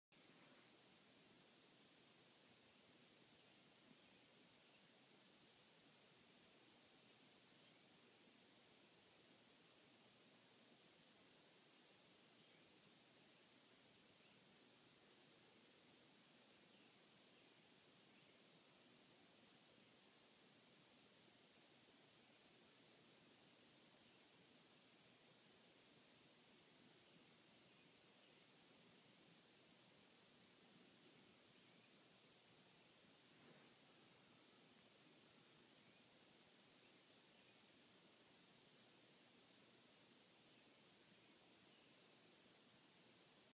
Cracked out lil bird chirping at 3:26am
Don't be giving it away for free, cracked out lil hooker bird. Save it til dawn.